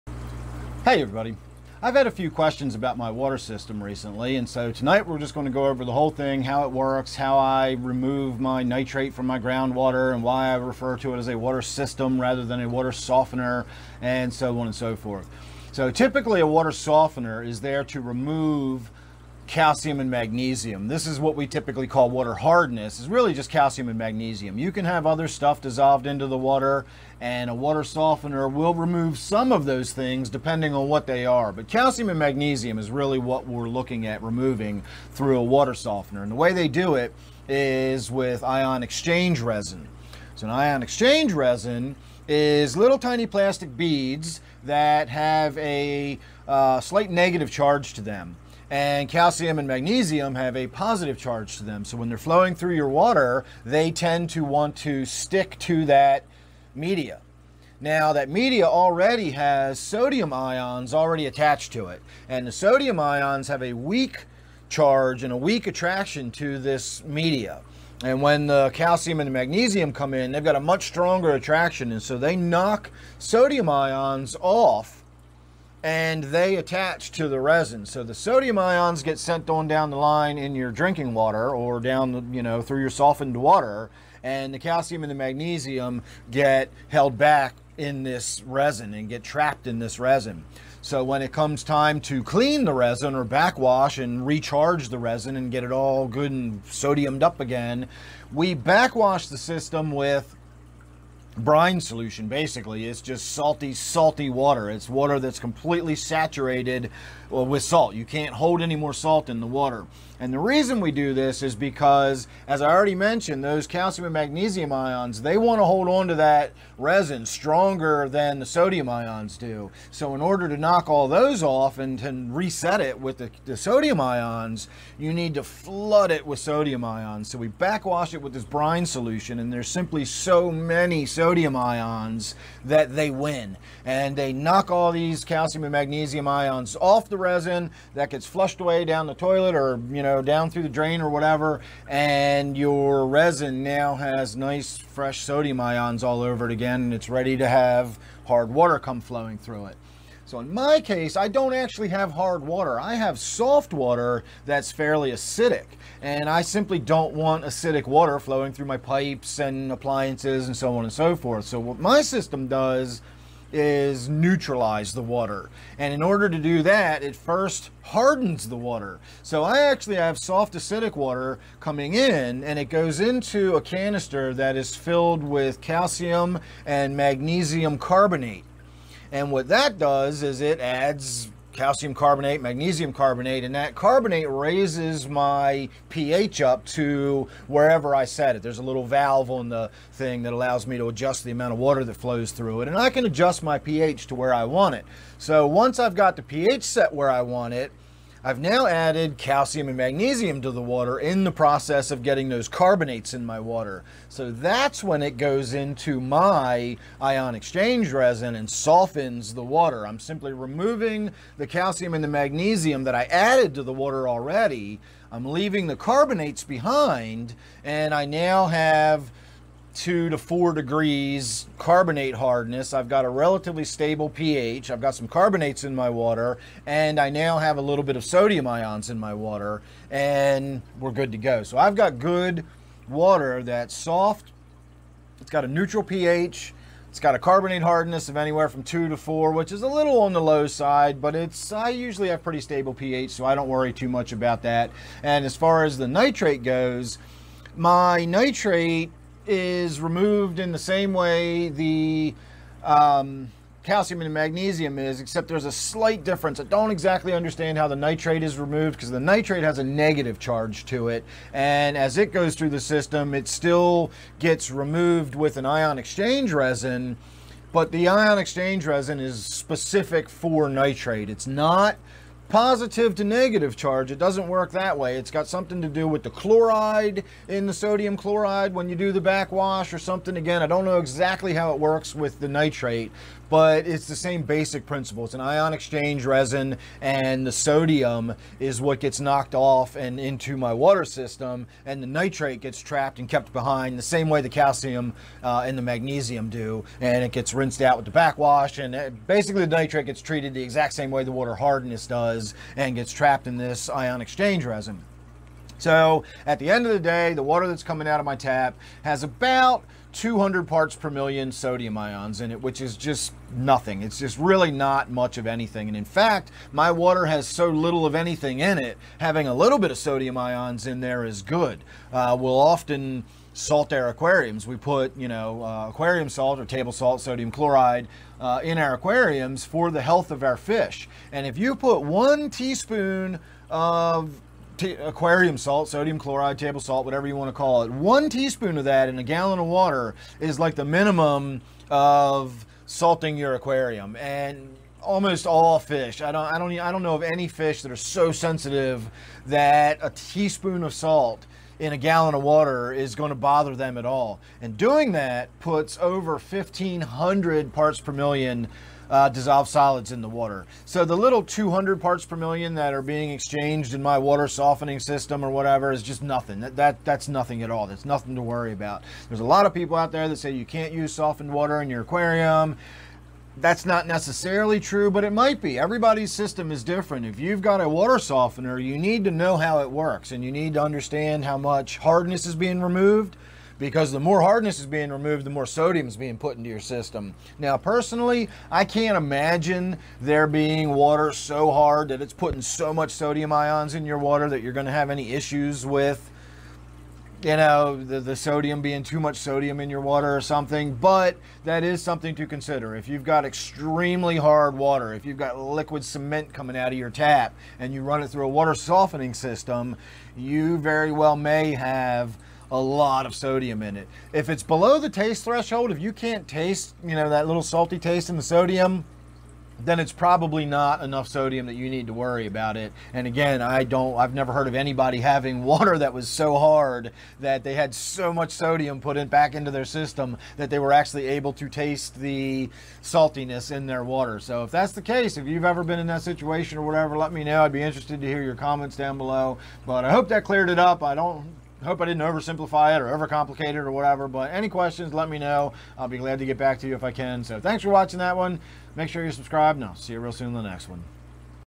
How I Remove Nitrate And Stabilize pH
This video explains how my water softener removes nitrate from my well water and neutralizes and stabilizes my pH.